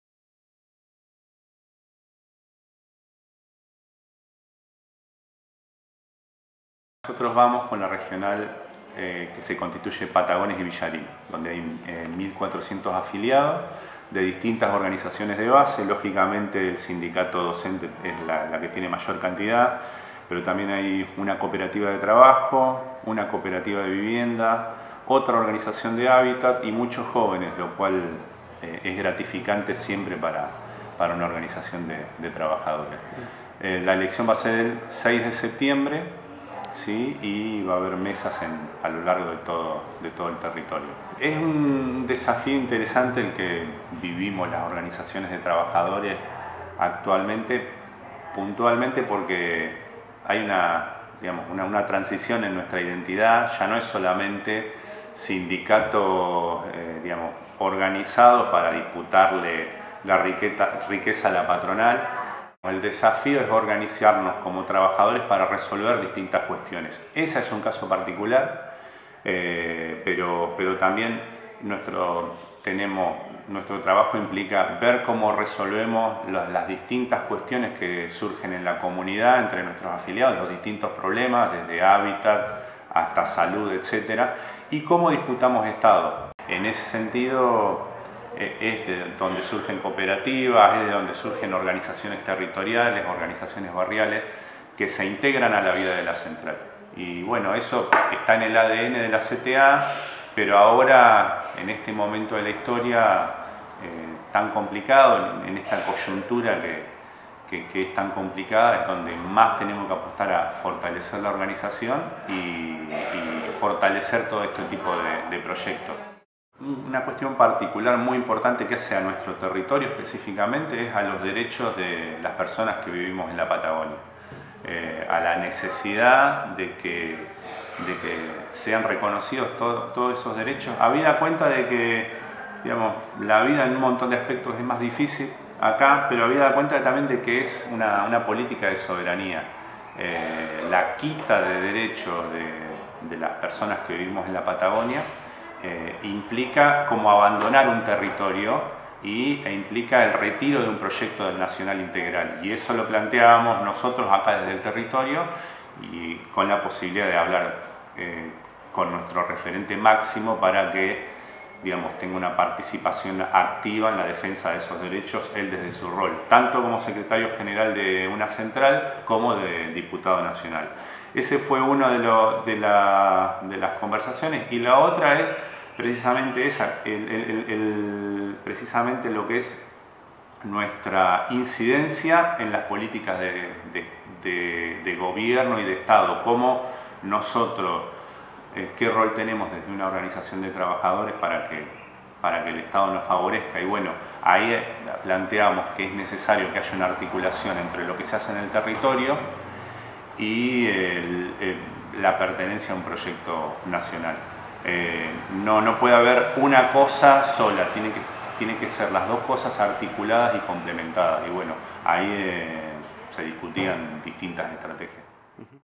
Elecciones en la CTA de los Trabajadores: Lista 10 en Patagones
El próximo 6 de septiembre se realizarán las elecciones en la CTA provincia de Buenos Aires y en todo el territorio nacional.